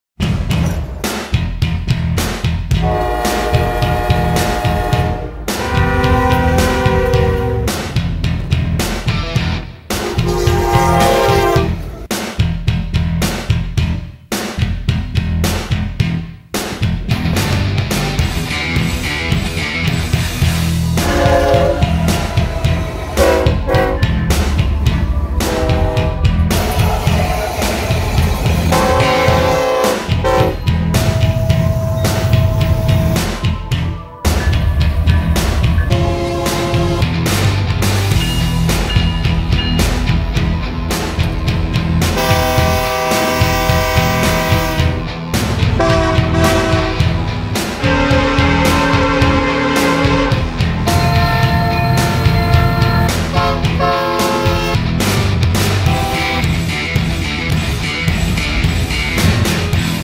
Super 7 Northern Rails Channel Intro
Venía en el camión de regreso a casa pensando en este soundtrack y esto fué lo que salió...
Intro para fijar en el canal :)